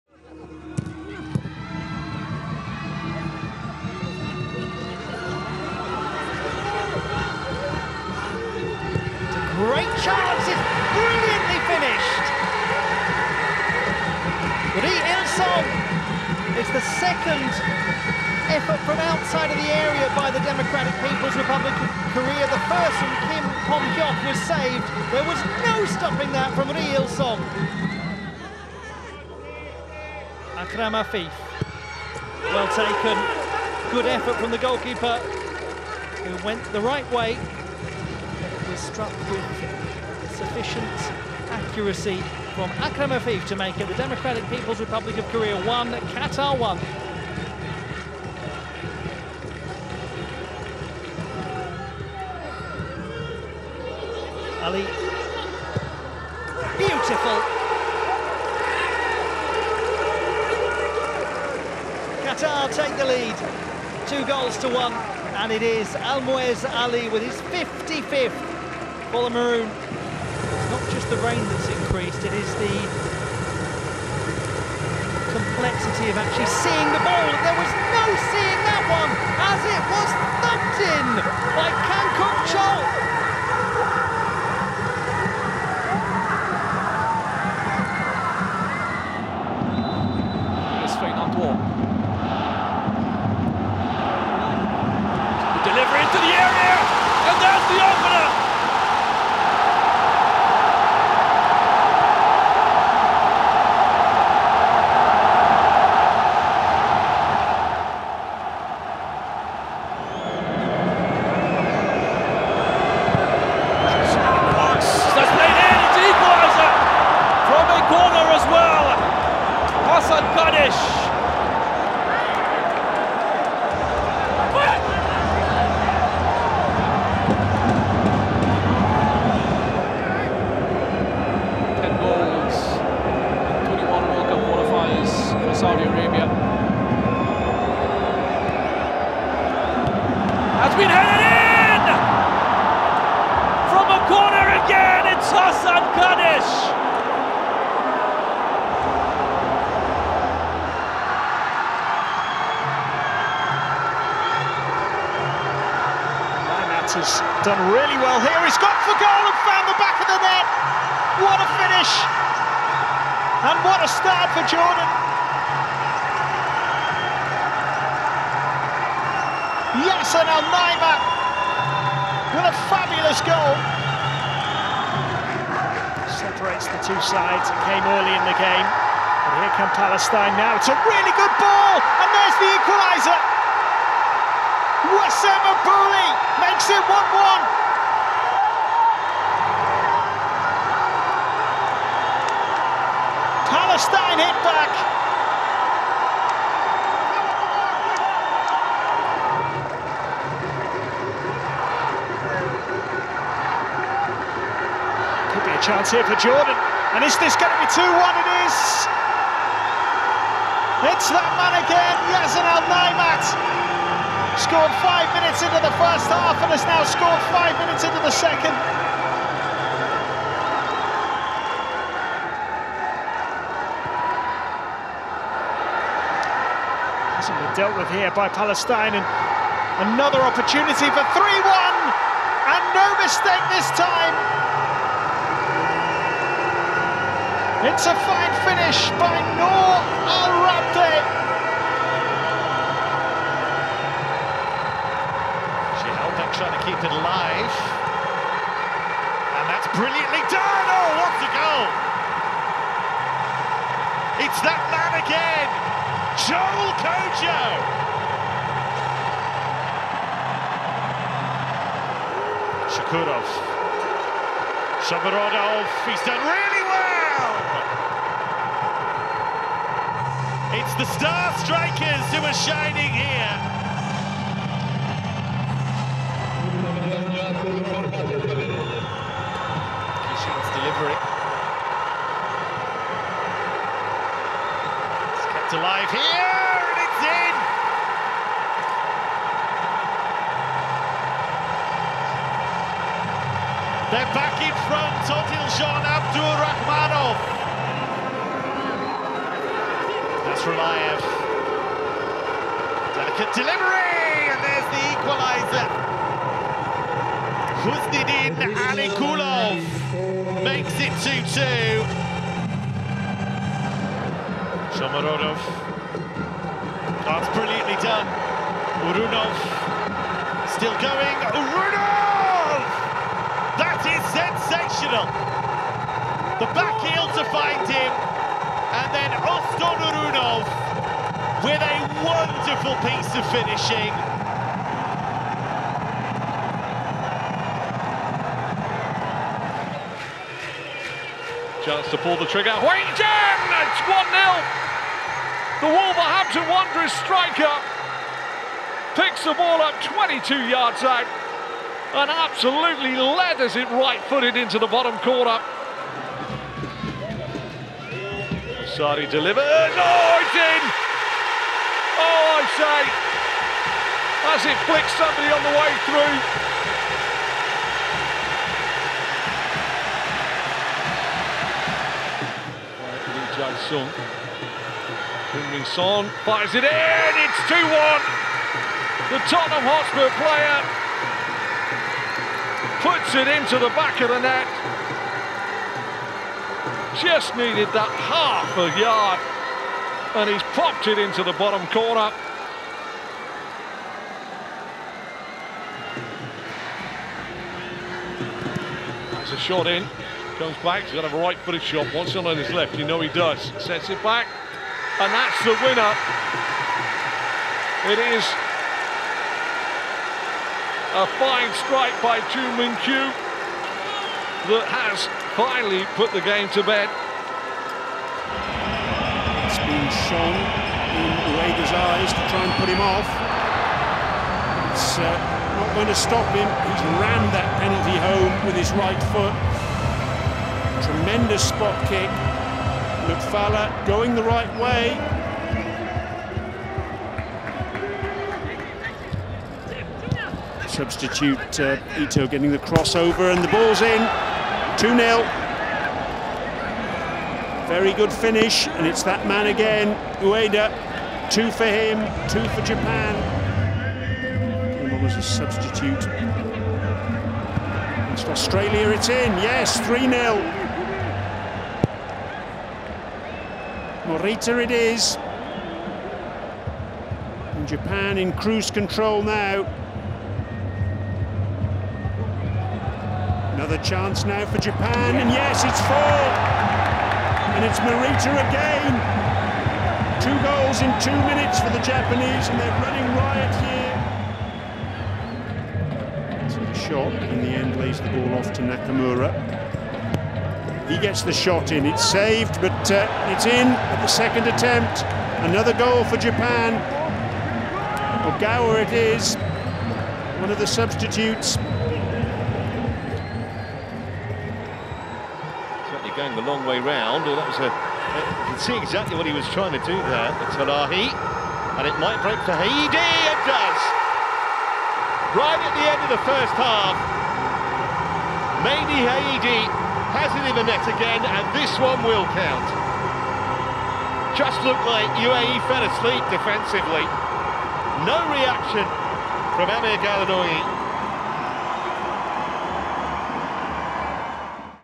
Incredible free kick from Kuk-chol! | Every Goal – Matchday 2 | AFC Asian Qualifiers™ Road to 26
Enjoy every goal from matchday 2 of the AFC Asian Qualifiers™ Road to 26, showcasing the best scoring moments so far! 🔥

Follow all the AFC Asian Qualifier™ action on the official platforms: 🏟️📲